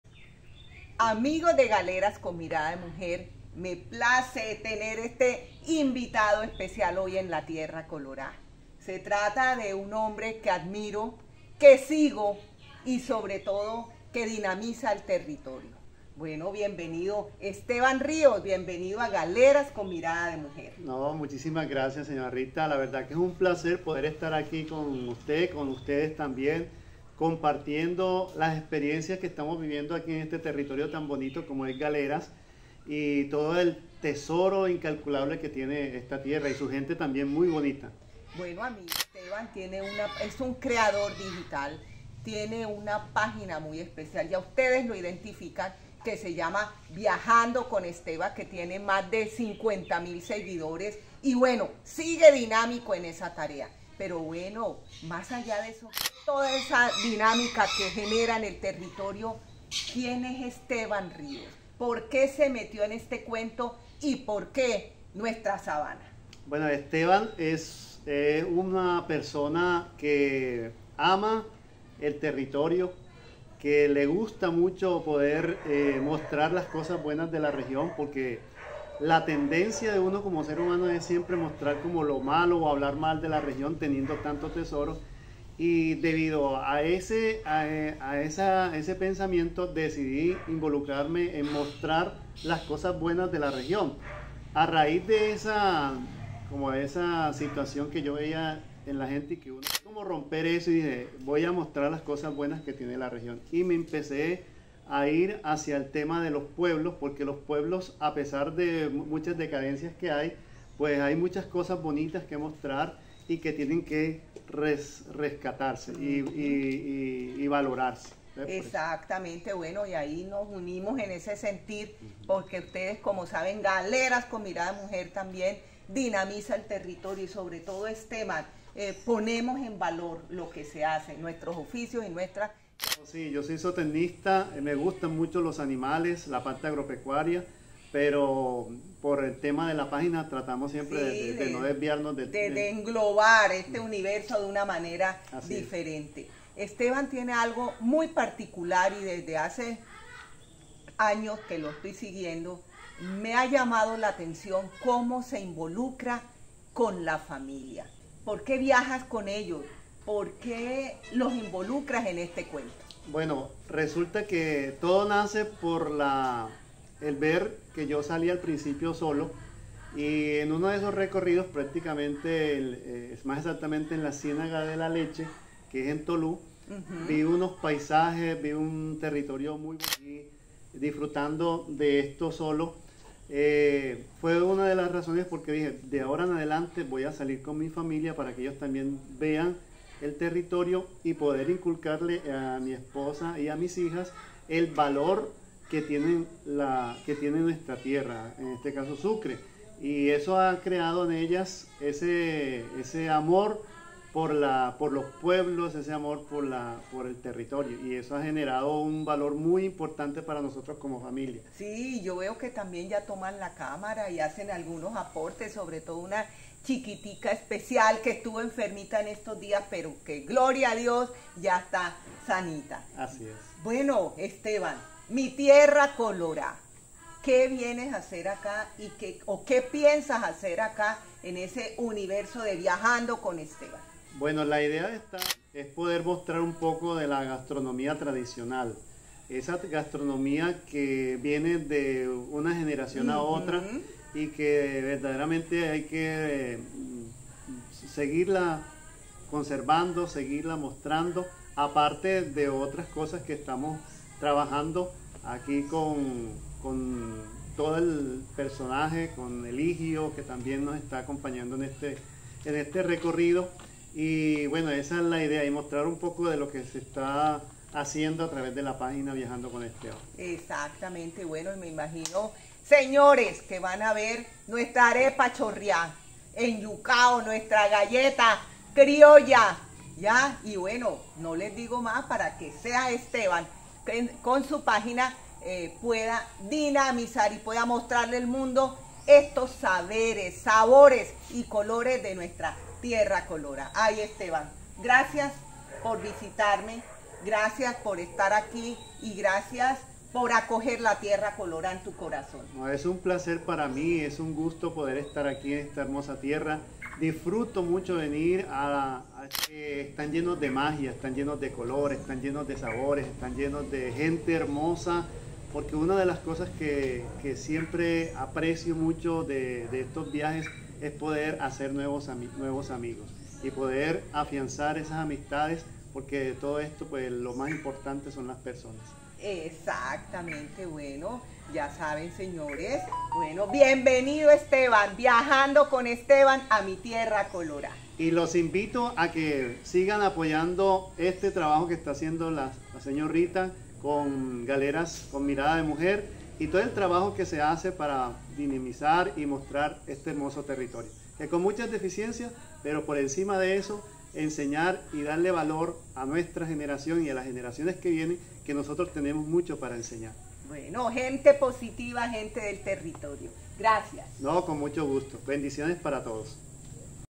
Esteban Rios " viajando con Esteban" en la tierra colorá.
Con carisma y creatividad llega a nuestra tierra y  colorá Viajando Con Esteban .

Esteban Rios, como creador digital, recorre caminos y resalta lo que somos,nuestra gente y las tradiciones .
Bienvenido!